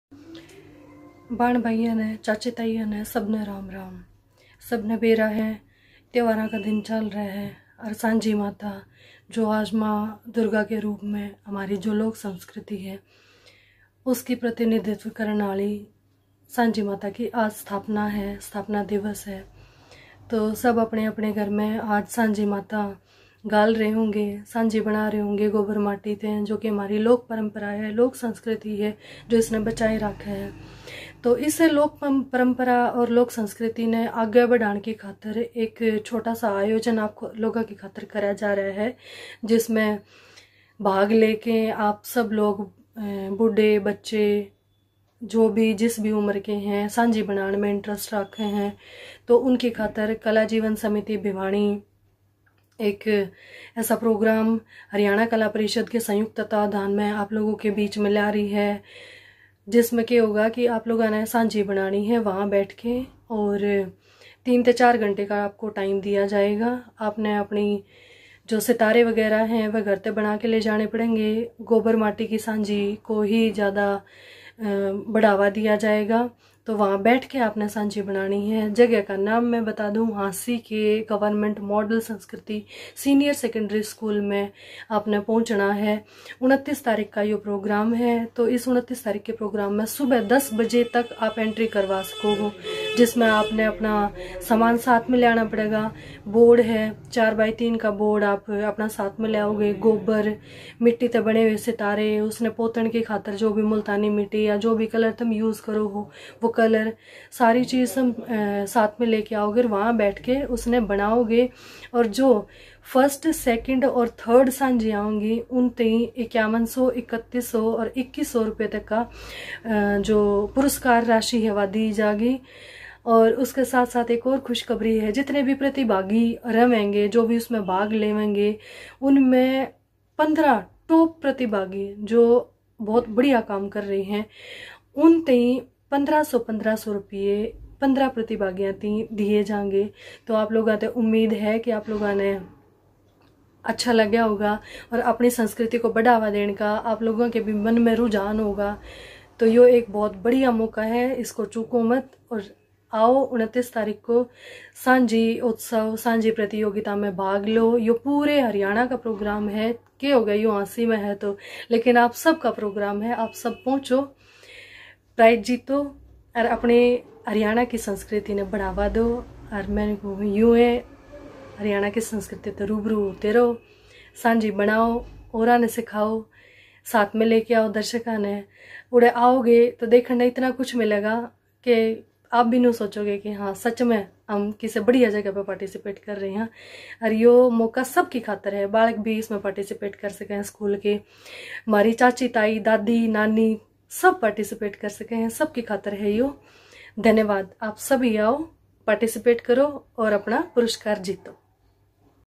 सांझी उत्सव प्रतियोगिता Sanjhi Competition
कला जीवन समिति,हरियाणा कला परिषद हिसार के संयुक्त तत्वाधान में गवर्नमेंट मॉडल संस्कृति सीनियर सेकेंडरी स्कूल हांसी मे दिनांक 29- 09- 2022 को हरियाणा की लोक कला सांझी प्रतियोगिता का आयोजन  करने जा रहे हैं आप सभी आमंत्रित हैं.